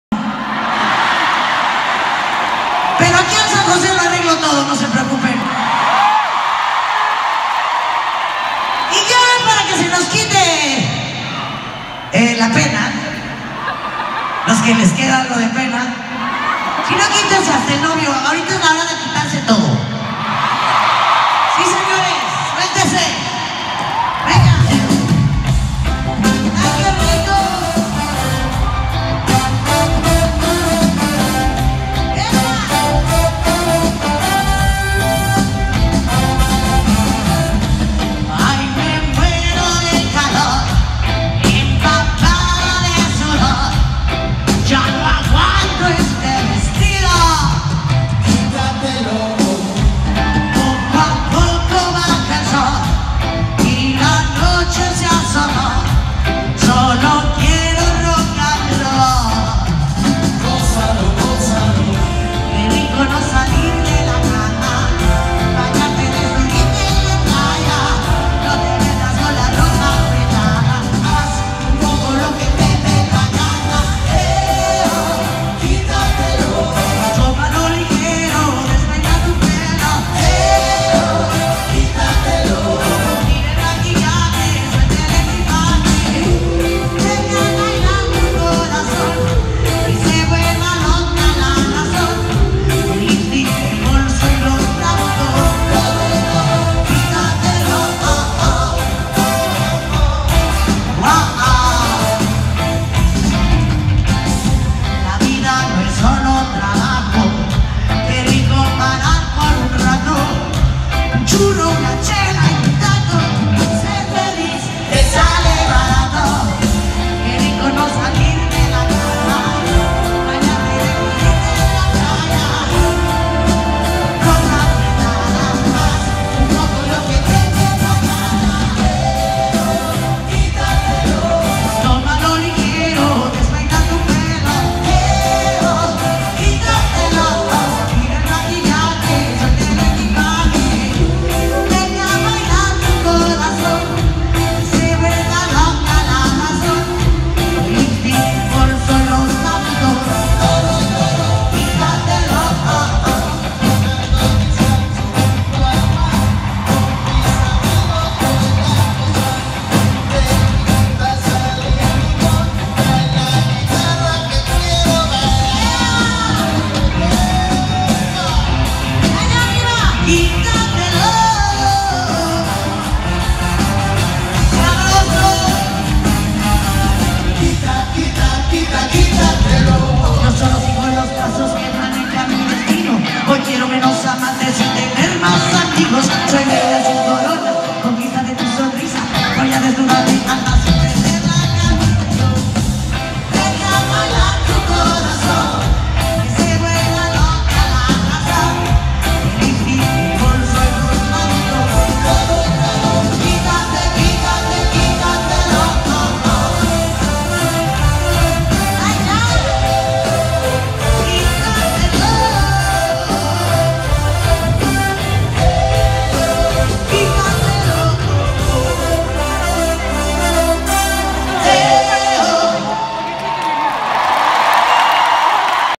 GLORIA TREVI & ALEJANDRA GUZMAN 06
ALEJANDRA GUZMAN & GLORIA TREVI TOUR
SAN JOSE, CALIFORNIA
SAP CENTER
SABADO 10 DE JUNIO DE 2017
EL CUARTO CONCIERTO DE LA GIRA DE GLORIA TREVI CON ALEJANDRA GUZMAN